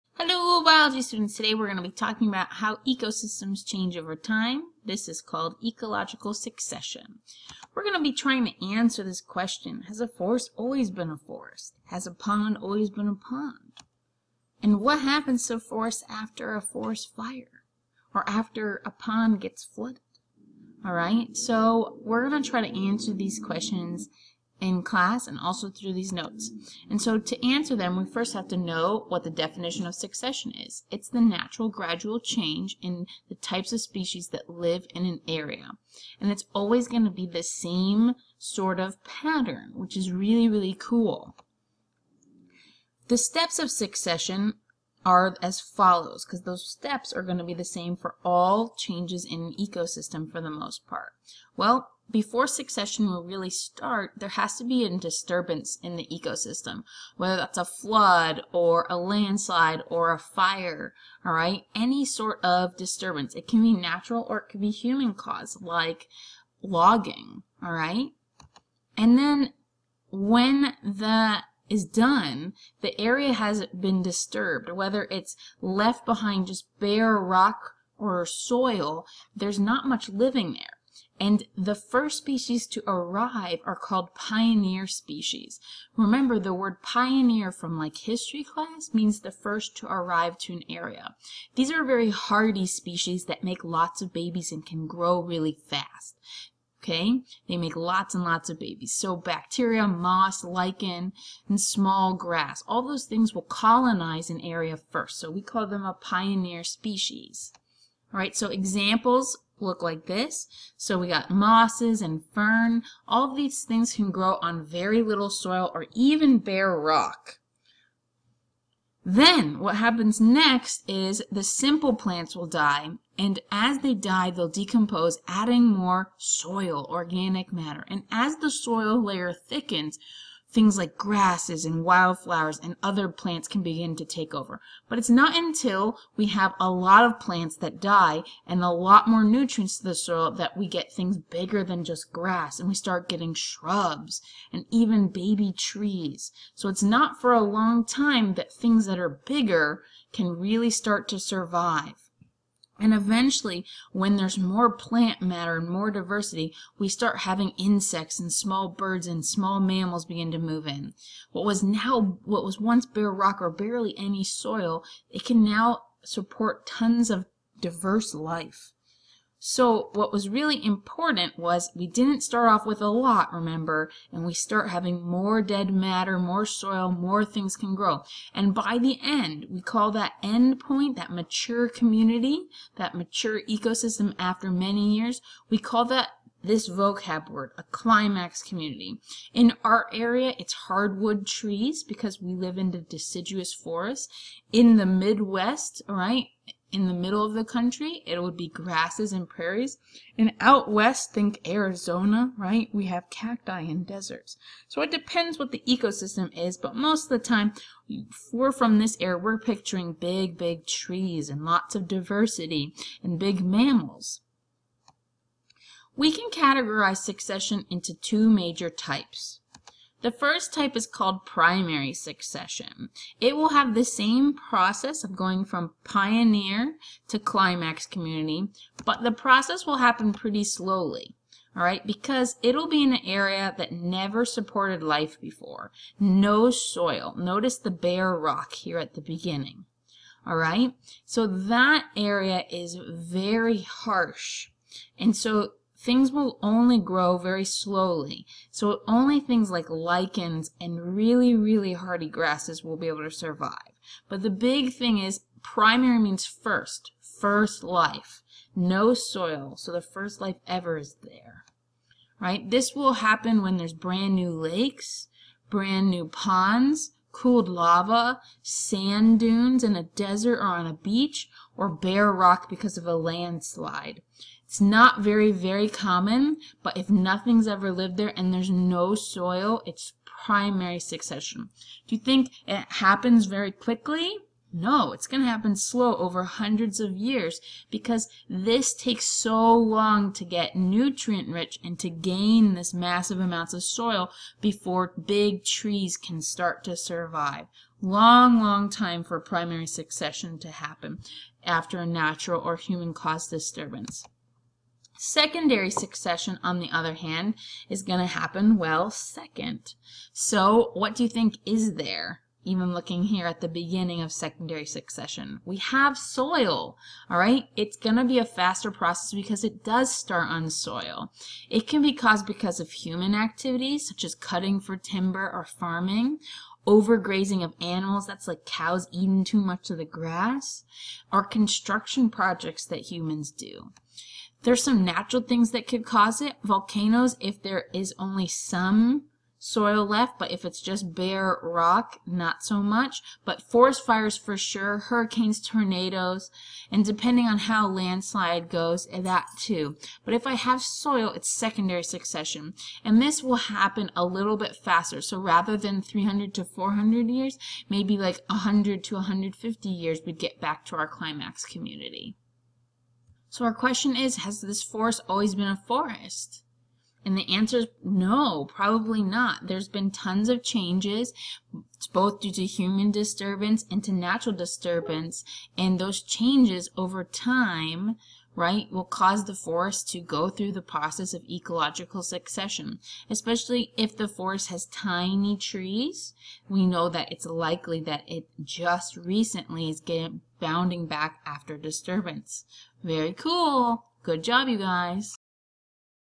Succession (Theme 1 Topic 1 pg 5)
Describe the major steps of ecological succession (disturbance, pioneer species, intermediate and soil growth, and lastly climax community).  Compare primary and secondary succession start point and speed.